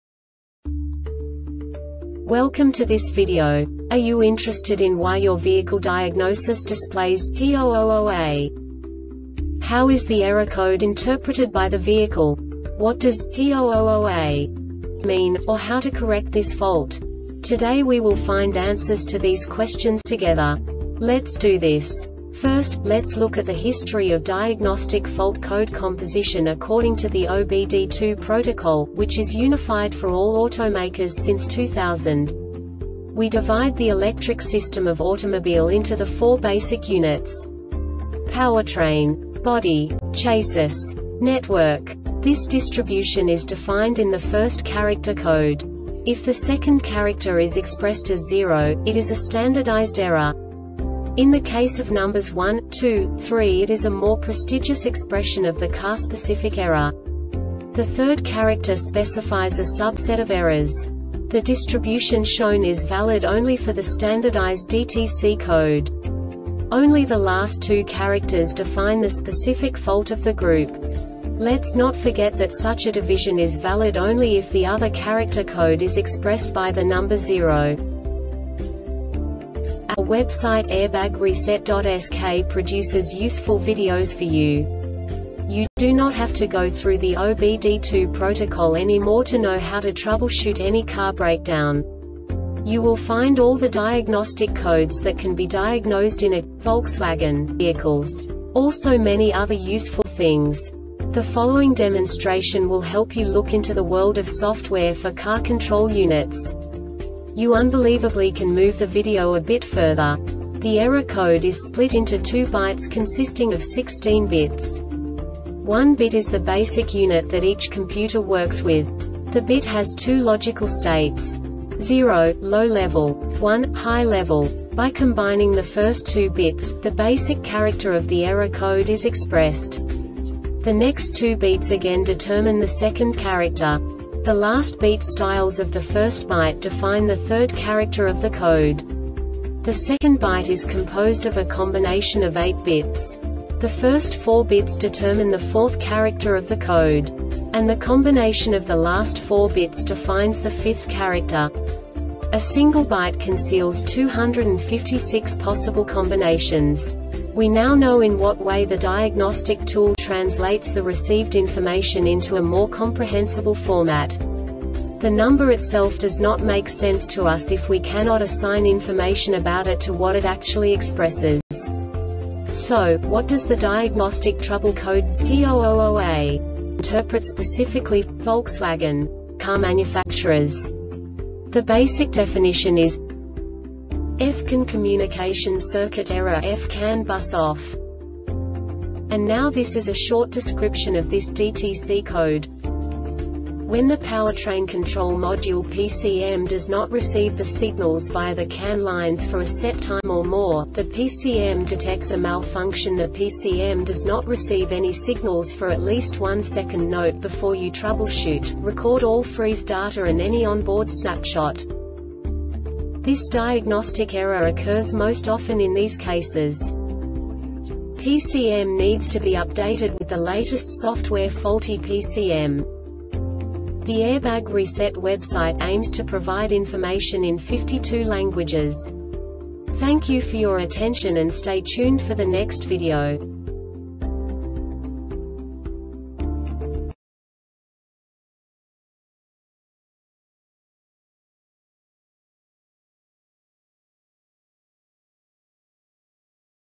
DTC Volkswagen P000A Short Explanation
Contents:
0:21 Basic DTC analysis according to OBD2 protocol standard.
1:48 Insight into programming
2:58 A diagnostically understandable description of the Diagnostic Trouble Code
3:05 Basic error definition

Make:
Volkswagen
Code:
P000A
Definition:
F-CAN Communication Circuit Error (F-CAN Bus OFF)
Description:
When the powertrain control module (PCM) does not receive the signals via the CAN lines for a set time or more,the PCM detects a malfunction.The PCM does not receive any signals for at least 1 second.NOTE: Before you troubleshoot,record all freeze data and any on-board snapshot.
Cause:
 PCM needs to be updated with the latest software Faulty PCM